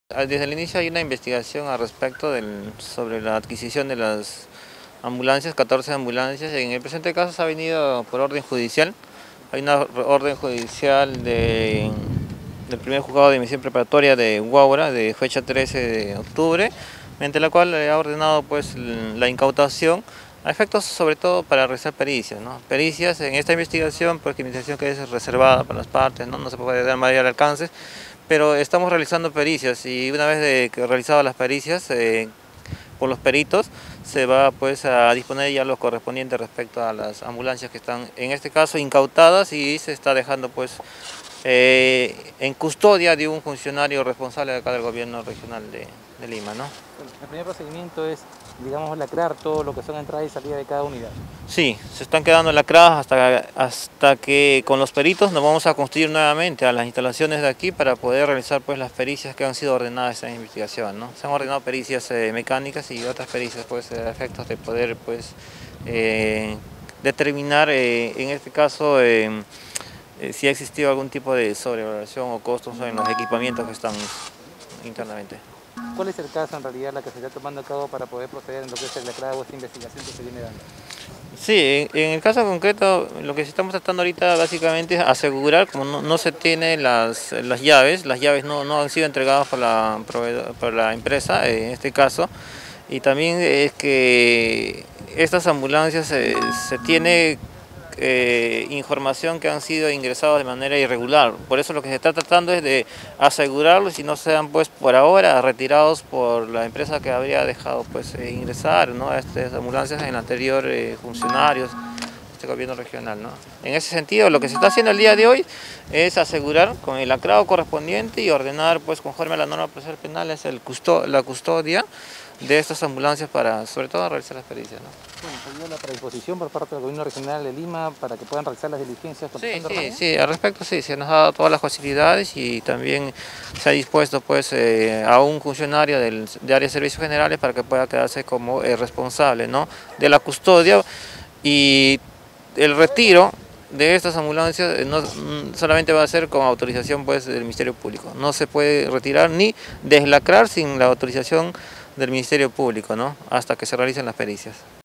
Con los peritos vamos a determinar si es que ha existido algún tipo de sobrevaloración
Con los peritos vamos a determinar si es que ha existido algún tipo de sobrevaloración en los equipamientos de las ambulancia